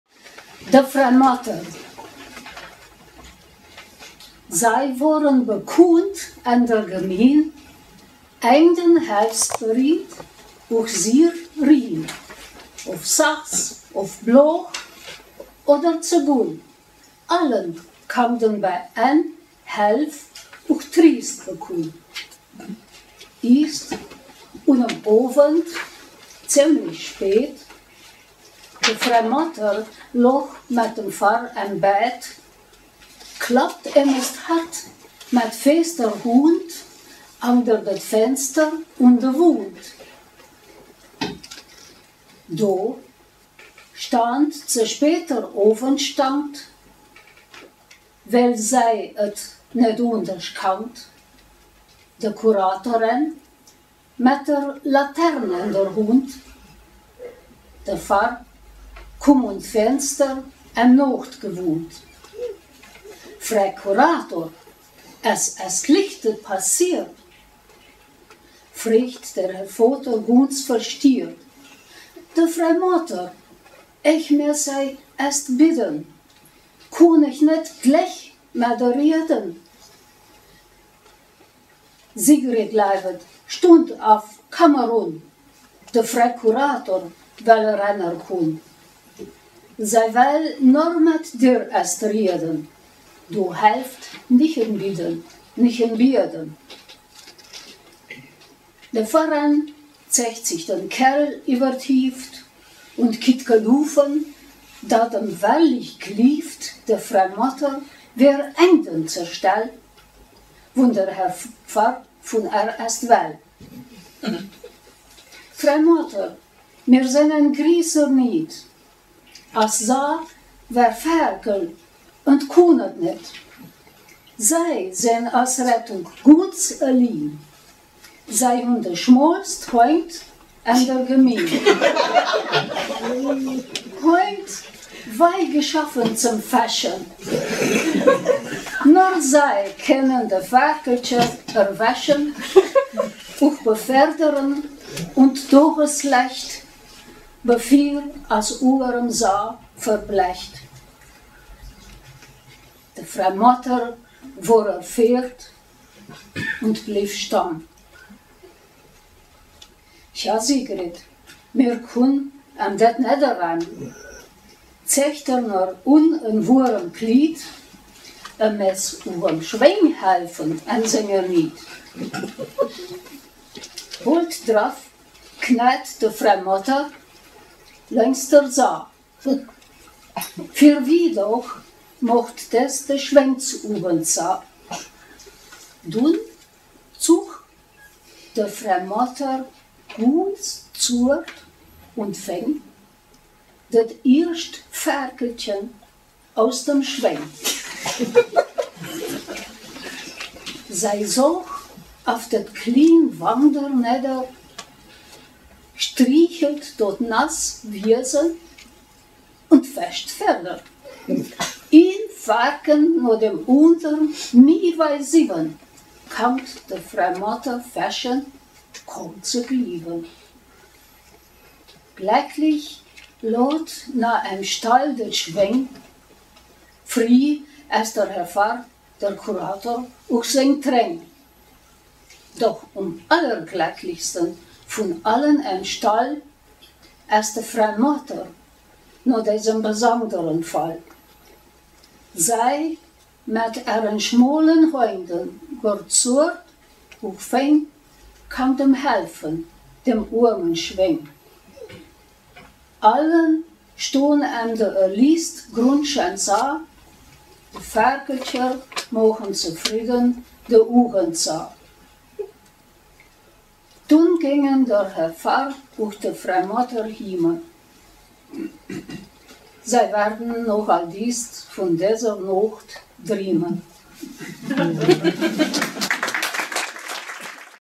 Elisabeth Kessler: "De Fra-Motter" (vun Rose Schmidt) - Mundartautorentreffen 2015
Elisabeth Kessler mit "De Fra Motter" beim Siebenbürgisch-Sächsischen Mundartautorentreffen 2015 im Haus der Heimat in Nürnberg.

Elisabeth Kessler, geb. Schwachhofer
Geboren 1951 in Neppendorf, gelebt auch in Hermannstadt, Verwaltungsangestellte, Aussiedlung 1990, wohnt in München. Veröffentlichungen u.a. in "Neue Literatur", Anthologie "Vill Sprochen än der Wält", "Wat mir schreiwen" und "Barbes" sowie im eigenen Gedichtband "Af deser Ierd als Gast derhiem".

De Fra-Motter (vun Rose Schmidt)

Säi wiuren bekunnt än der Gemin,
ängden helfsberiet och siehr rien.
Of Sachs, of Bliuch awer Zegun,
allen kangden bäi än Hälf och Triust bekunn.

Ist un em Iwend, zemlich spet,
de Fra-Motter lauch mät dem Pfarr äm Bett,
klopt ämest hort, mät fester Hund
angder det Fenster un de Wund.

Diu stiand ze speter Iwendstangt,
wel säi et net undersch kangt,
de Kuratoran mät der Lataren än der Hund,
der Pfarr kum un't Fenster, äm Noichtsgewund:

"Fra-Kurator, äs äst lichtet passiert?",
frecht der Herr Voter gunz verstiert.
"De Fra-Motter…, ech muaß säi äst bidden,
kunn ech net glech mät är riaden?"

"Sigrid läwed, stund af, kamm erun,
de Fra Kurator wäl eräner kunn.
Säi wäll nor mät dir äst rieden
diu hälft nichen bidden, nichen biaden."

De Pforran zoicht sich den Kerrel iwer det Hift
und kit gelufen, dat em wahrlich gliewt
de Fra-Motter wer ängden zer Stall
wun der Herr Pfarr vun är äst wäll.

"Fra-Motter mir seng än guißer Nuit
as Sia wäll färkeln, en kunn et net,
säi seng as Rettung gunz ellin,
säi hun de schmolst Hoingd än der Gemin.

Hoingd wäi geschaffen zem "fäschen",
nor säi kennen de Färkeltscher erwäschen
och beferderen unt Daujeslächt,
bevuar as orem Sia verblächt.

De Fra-Motter wor erfert, ant bliw stamm.
"Ja, Sigrid, mir kunn am det net eram,
zoich dir nor un en worem Klied
em muaß och em Schweng helfen än senger Nuit.

Bauld draf knäit de Fra-Motter loingst der Sia,
vuar Wiehdouch maucht des är Schwengsujen zia,
dunn zuch de Fra-Motter gunz zort och feng,
det irscht Färkeltschen eus dem Schweng.

Säi sauch af det klien Wangder negder,
strichelt daut naß Wiasen und fäscht ferder,
in Färken niu dem undern, mie wäi siwen
kangt de Fra-Motter fäschen, kem ze gliewen.

Gloacklich läht nia äm Staul det Schweng,
frui äs der Herr Pfarr, der Kurator och seng Treng,
doch um gloacklichsten vun allen äm Staull
äs de Fra-Motter niu desem besangderen Faull.

Säi mät ärer schmolen Hund, gor ziort och feng
kangt em hälfen, dem ormen Schweng.
Allen stiuhn am de erliest gruntschan Sia,
de Färkeltscher mauchen zefrieden de Ujen zia.

Dunn gängen der Herr Pfarr och de Fra-Motter hiemen.
Säi warden noch alldiest vun deser Noicht driemen.

Gefördert aus Mitteln des Bayerischen Staatsministeriums für Arbeit und Soziales, Familie und Integration.